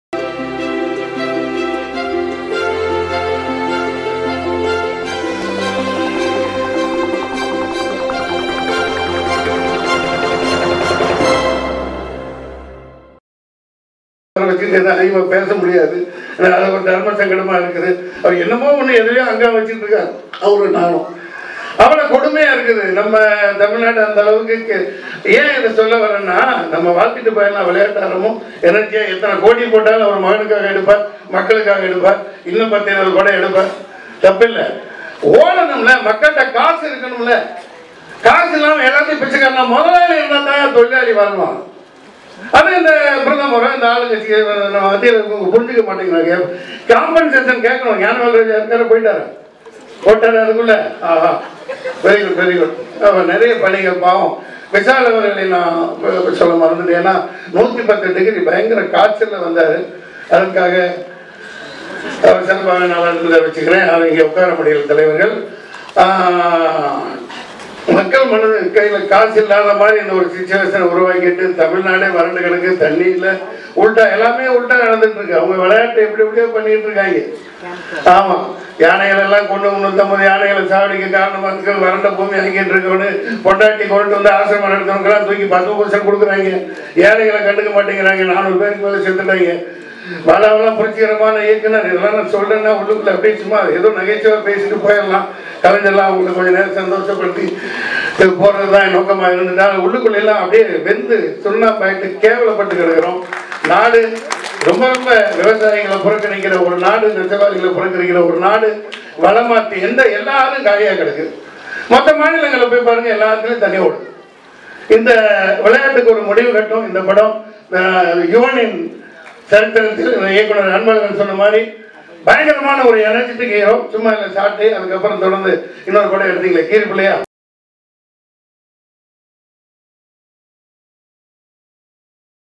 மோடி யை கழுவி ஊற்றிய மன்சூர்அலிகான் கலக்கல் பேச்சு | Vishal | Bala| CExpress
மோடி -ஐ கழுவி ஊதிய மன்சூரலிகான் கலக்கல் பேச்சு

Vilayattu Aarambam Movie Audio Launch | Vishal | Bala| CExpress